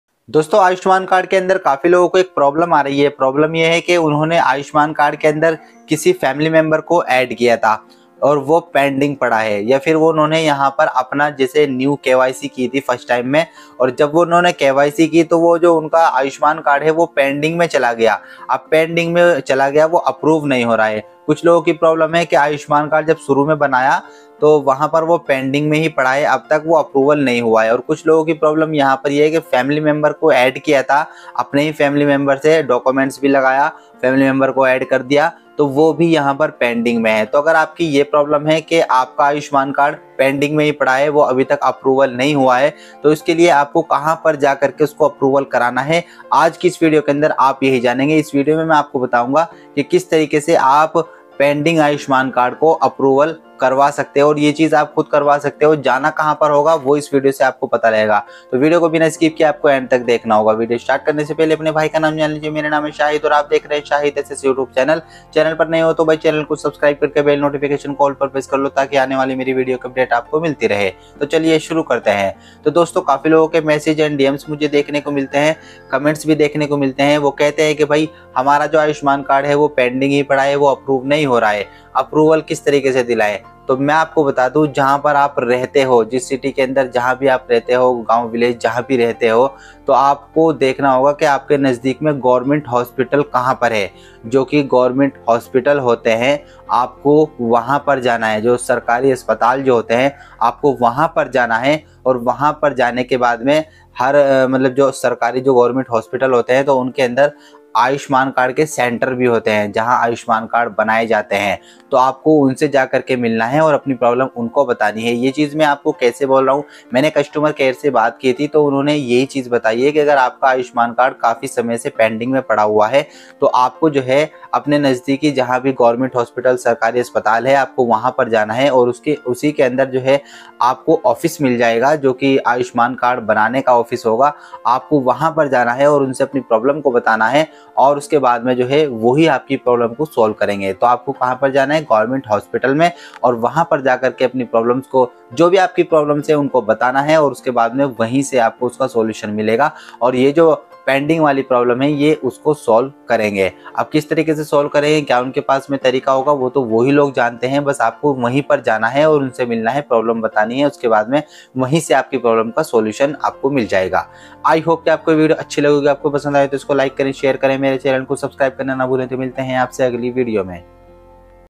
ayushman card pending problem | ayushman card pending approval | ayushma card pending approved
Dosto is video me hamne apko bataya hai aap ayushman card pending problem ko kaise solve karwa sakte ho kaise approved kara sakte ho

My Telegram Channel Link Join Karo